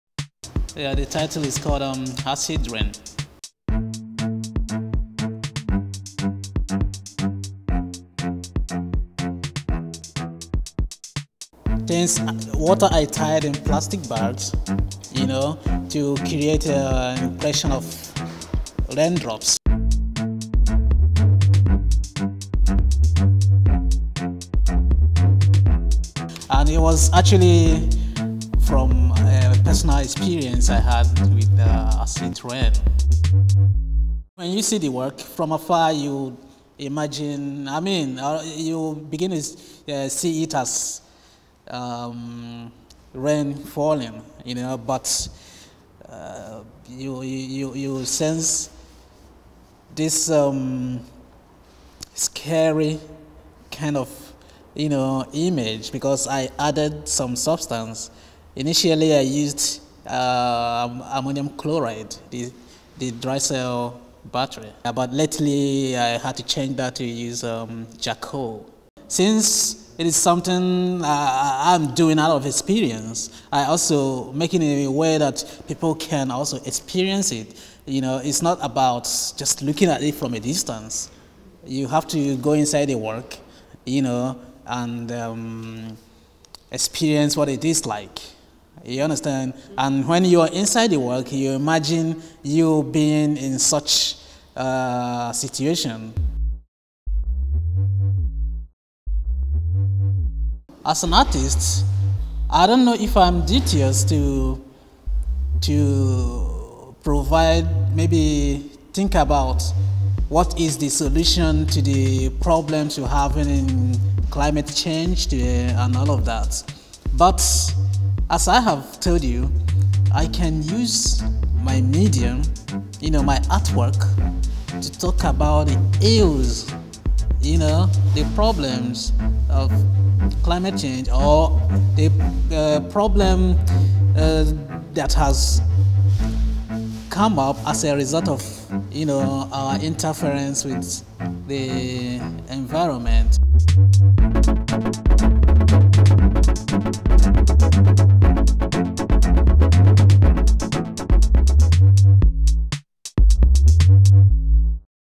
Bright Eke's Acid Rain @Den Frie, Copenhagen 2009
Interview with Bright Ugochukwu Eke, onsite at the exhibition RETHINK - Contemporary Art & Climate Chage